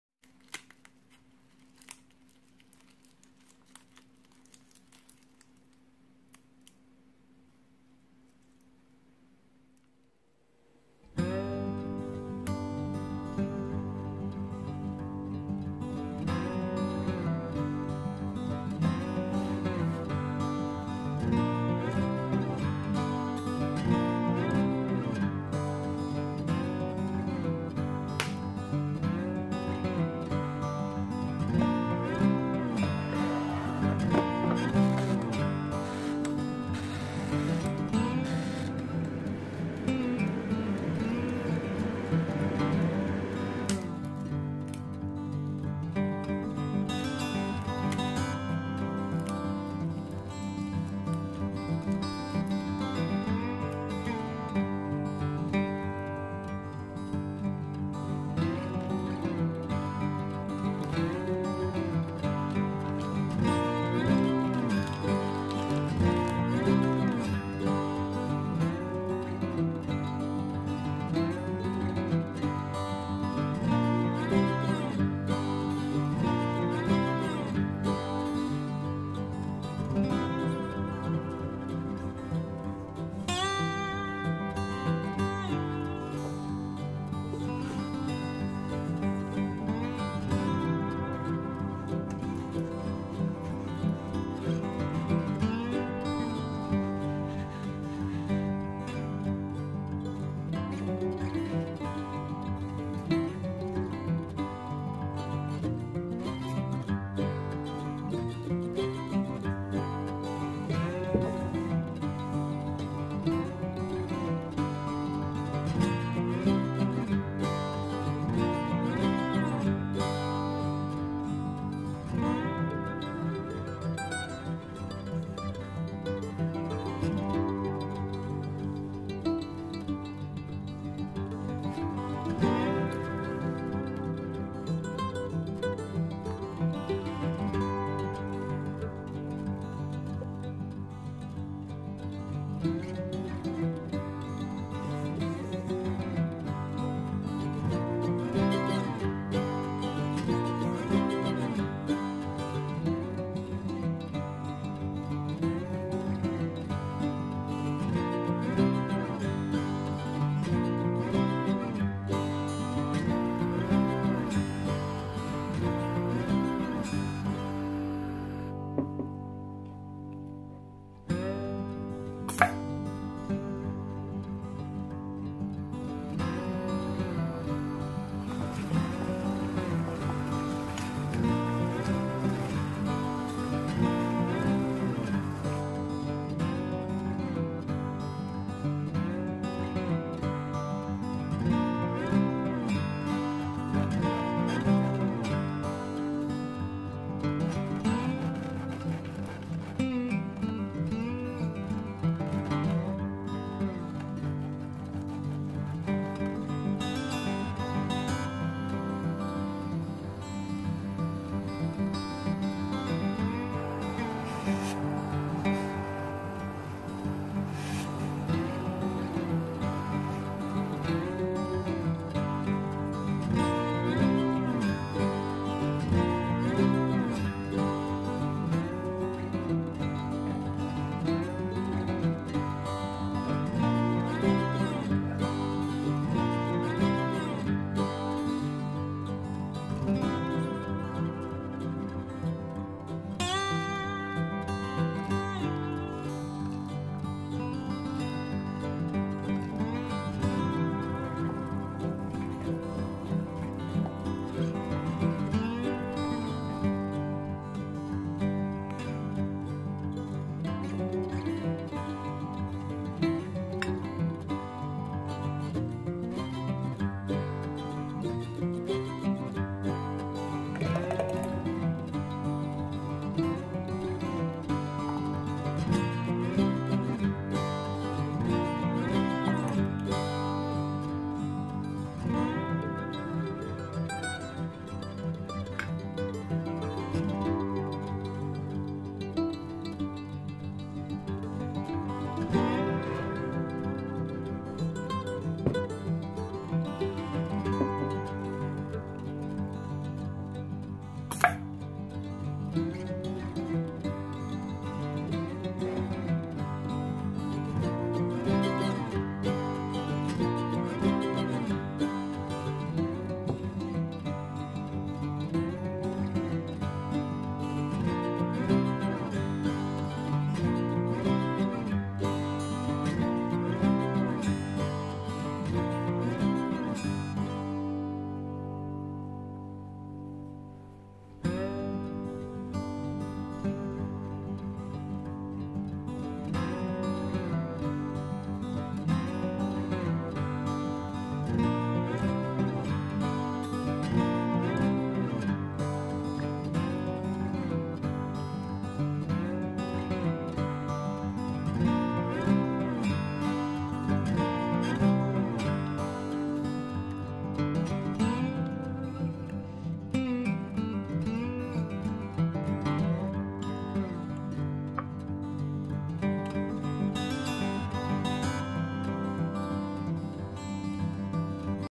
겨울 고구마와 귤로 맛있는 간식 만들기(군고구마 백설기, 귤쨈) JMT
같이 먹으면 궁합이 좋은 고구마. 귤.
겨울내 너무 많이 먹어서 질렸거나! 집에 처치 곤란한 고구마와 귤을 이용해서 군고구마설기와 귤잼을 만들어 보았어요~
따로 먹어도 맛있고 같이 먹어도 맛있는 군고구마설기와 귤잼 집에서 한 번 만들어 보세요~ ^^